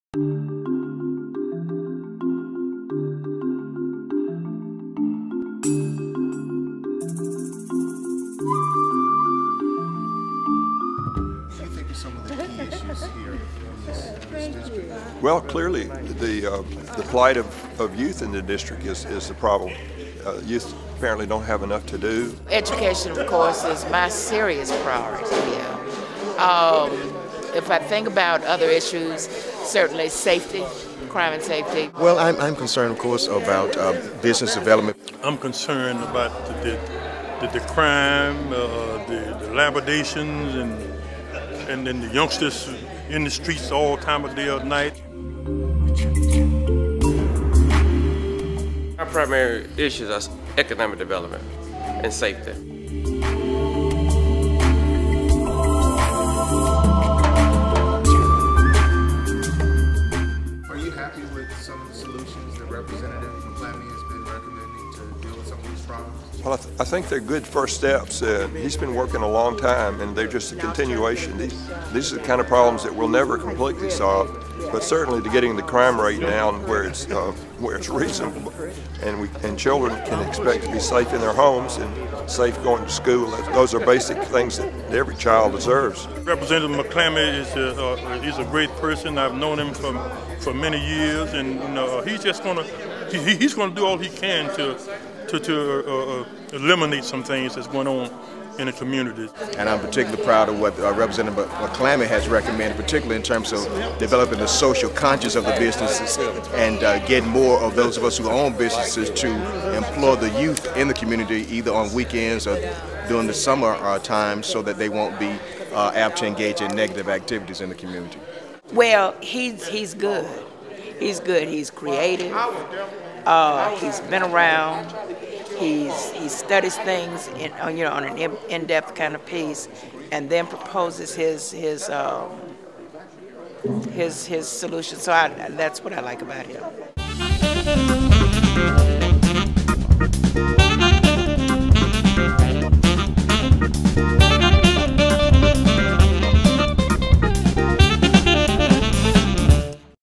Rep. Thad McClammy for AL Dist. 76 Re-Election Campaign Launch Reel
Campaign kick-off and launch reel for State Representative Thad McClammy of Alabama's District 76.  The primary election is June1, 2010.